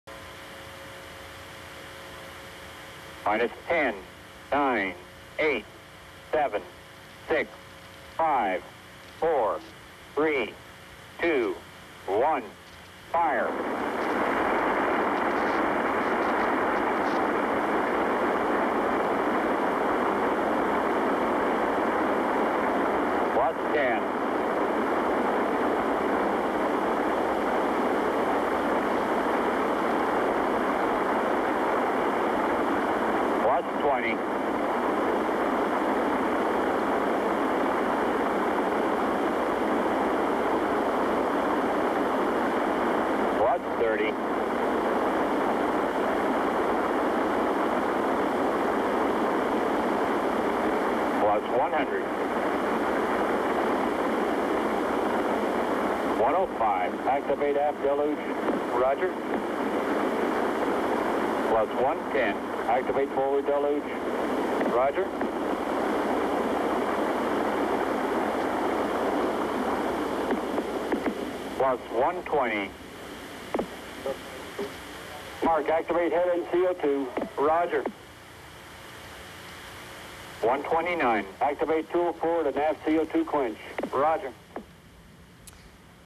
STS-26 Return to Flight SRB test
STS-26 Return to Flight Solid Rocket Booster firing test, the first after Challenger. Date unknown.